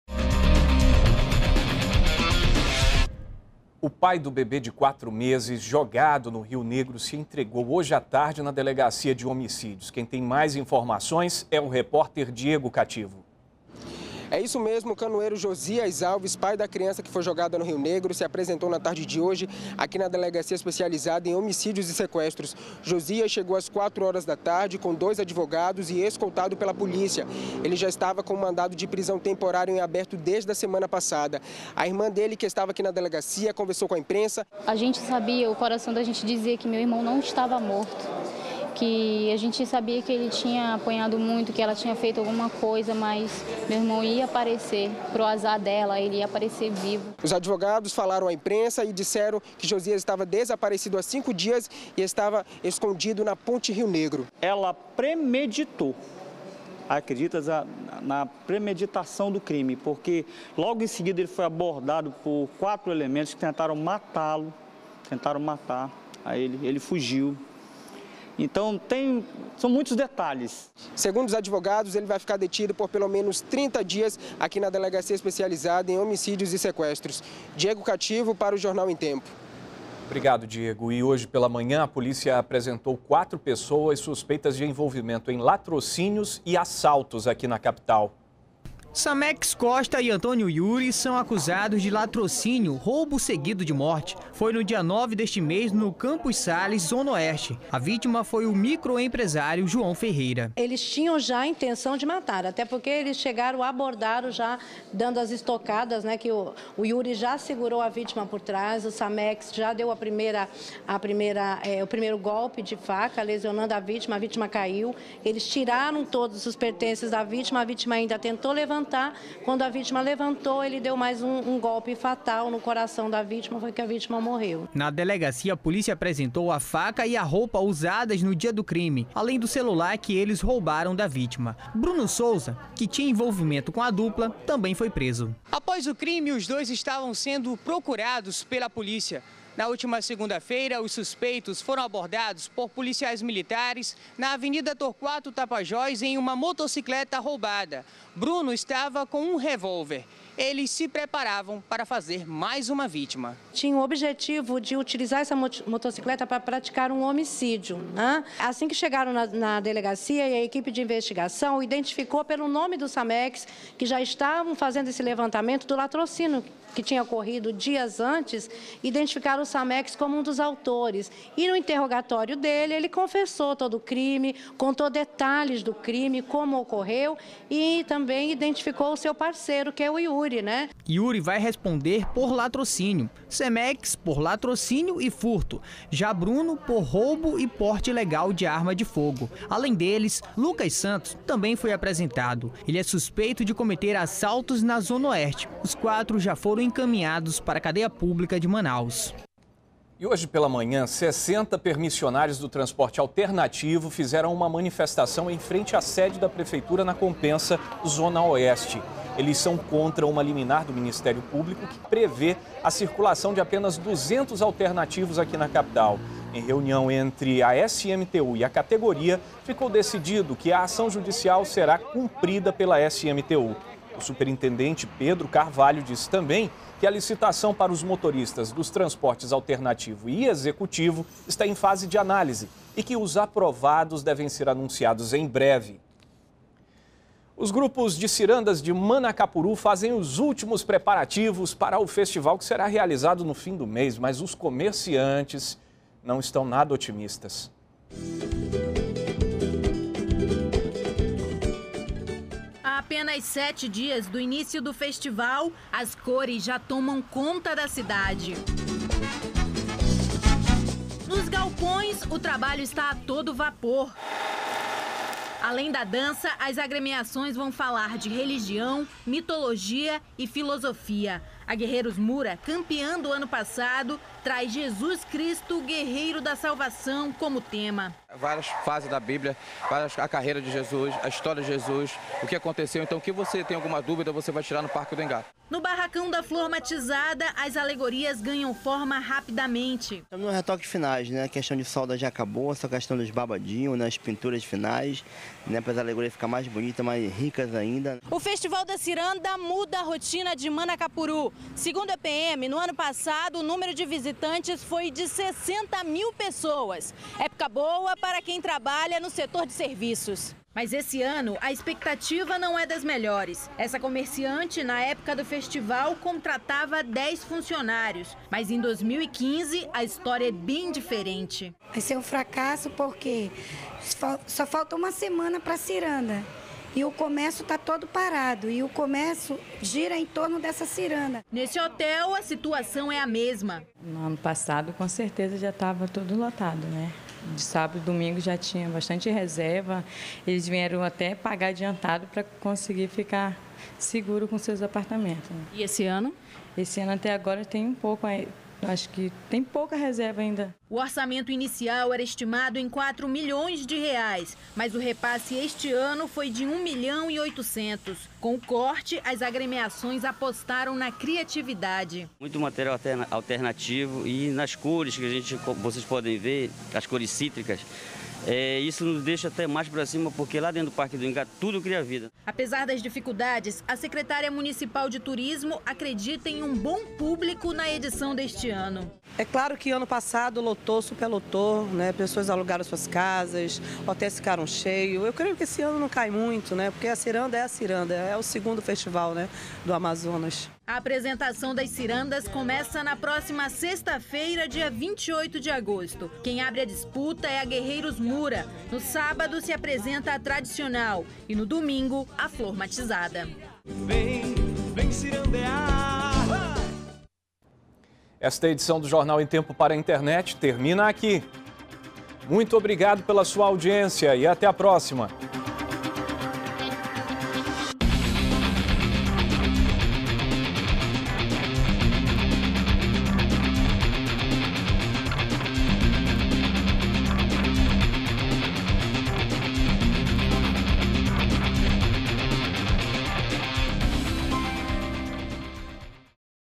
Pai do bebê de 4 meses jogado no rio negro se entrega na delegacia de homicídios
03º BLOCO JORNAL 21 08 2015 - Pai do bebê de 4 meses jogado no rio negro se entrega na delegacia de homicídios